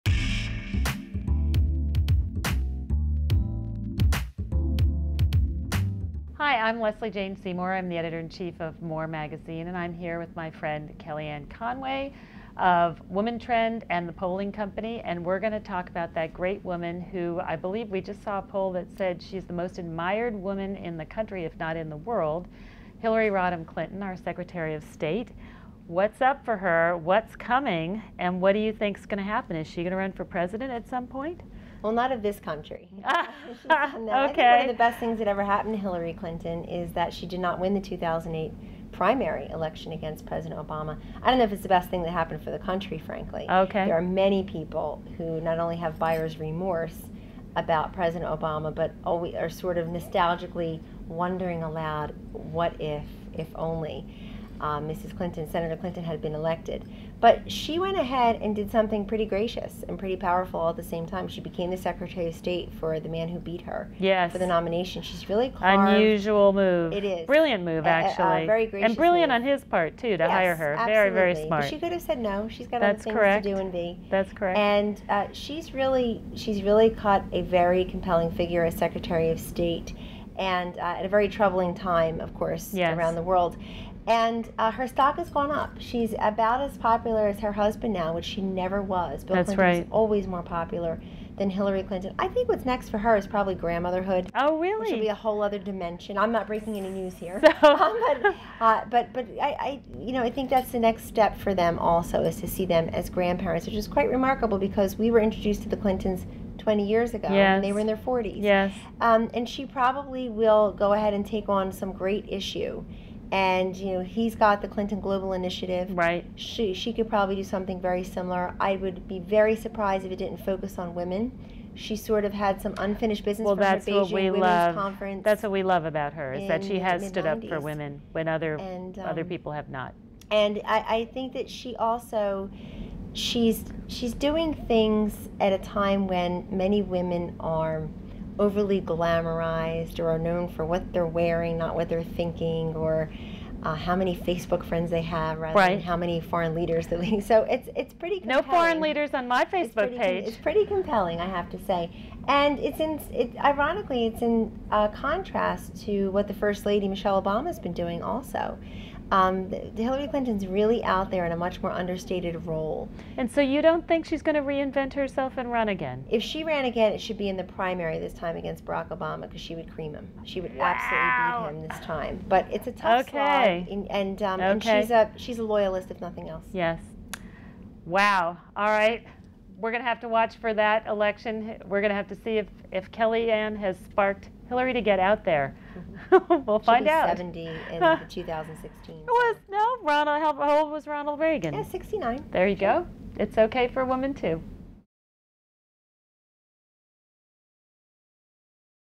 Two MORE Minutes: Will Hillary Clinton Run for President of the U.S.?
Editor-in-Chief Lesley Jane Seymour chats with Kellyanne Conway of the polling company inc. about the impact Hillary Clinton has had in politics so far despite losing against President Obama in the 2008 primaries.